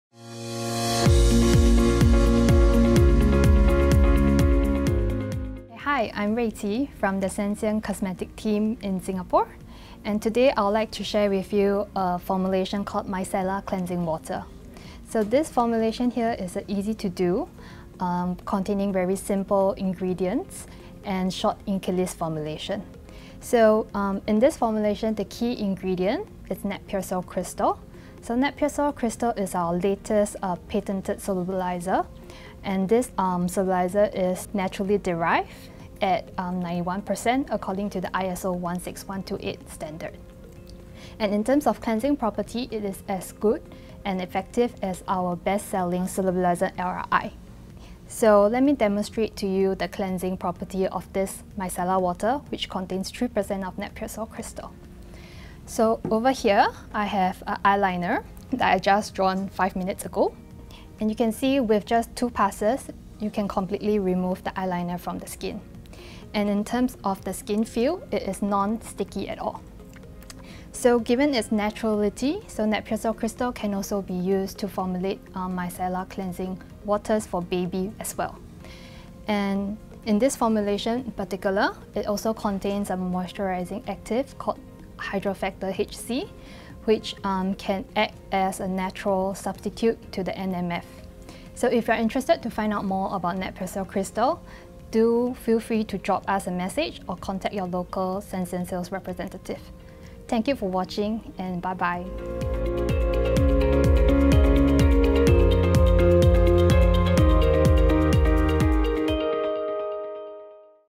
Formulator's Insight - Micellar Cleansing Water SCT2255
Join our application scientists as they explain more about the formulation and ingredients of Micellar Cleansing Water SCT2255